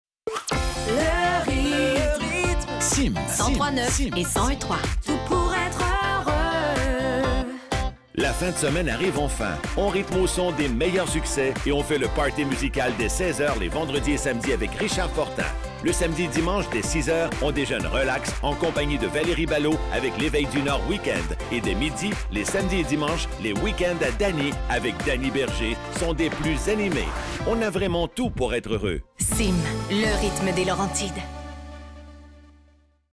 Cime 103,9 et 101,3 FM
Publicité de Cime des Laurentides diffusée sur les écrans du Cinéma Carrefour du Nord.